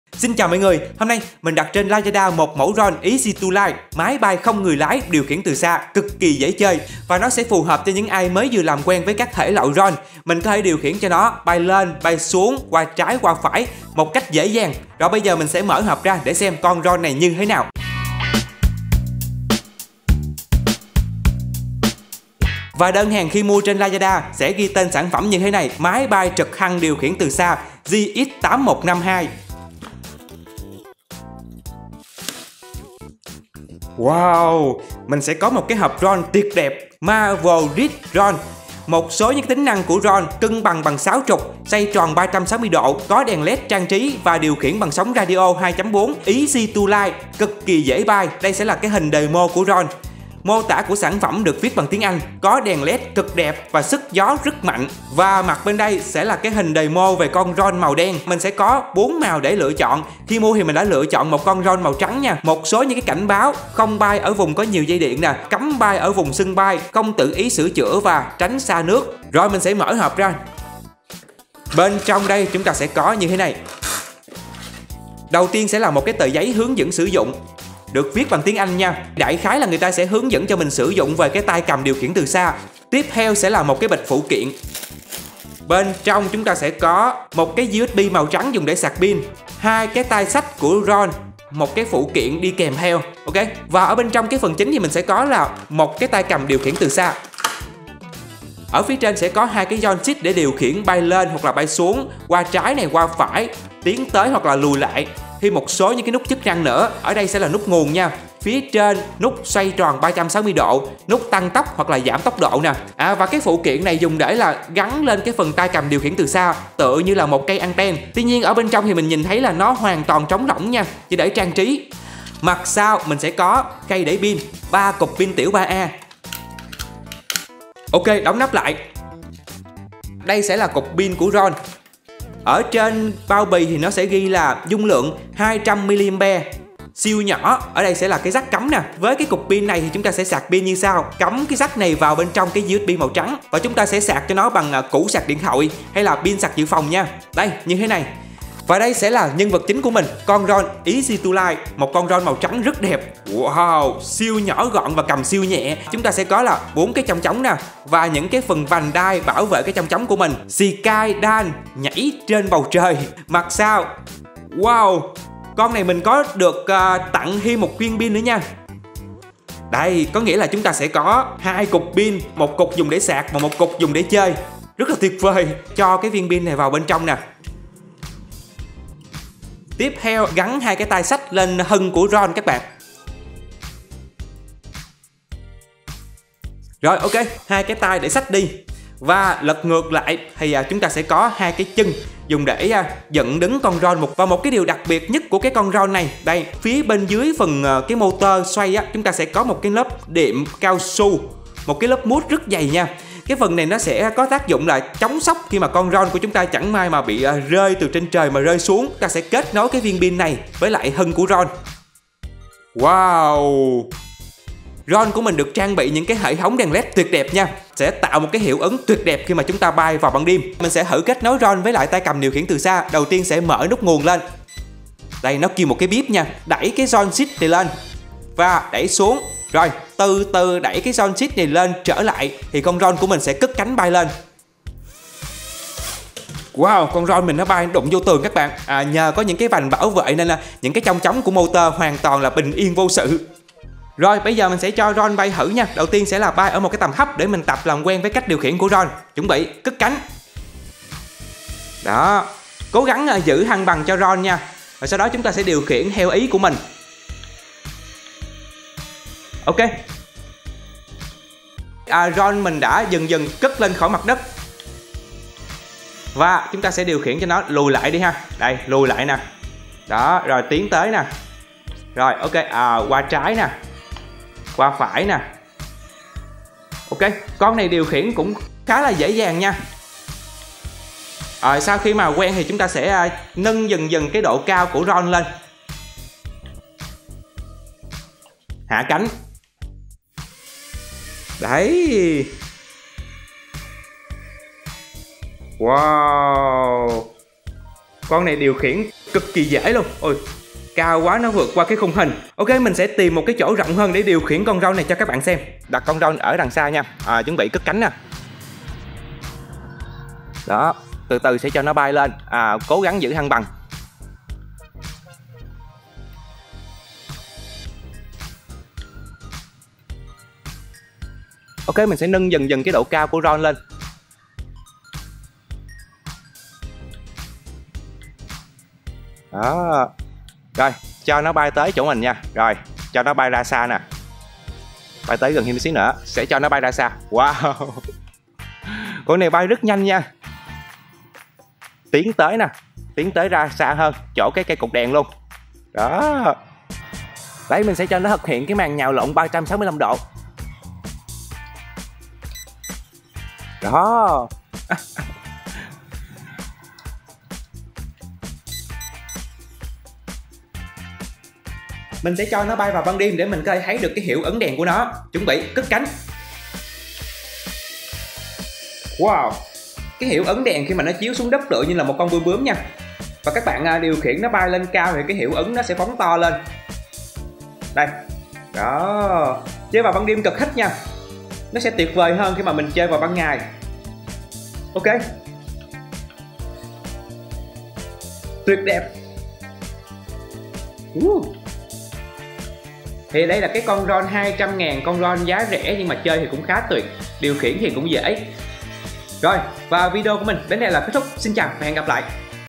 Drone Dễ Lái Cơ Bản, Máy Bay Không Người Lái Điều Khiển Từ Xa, JX815-2 | Mở Hộp Hàng Mua Trên Lazada
Xin chào mọi người. Hôm nay mình sẽ mở hộp một con drone cơ bản, dễ lái tập chơi cho người mới bắt đầu. Cám ơn vì đã xem.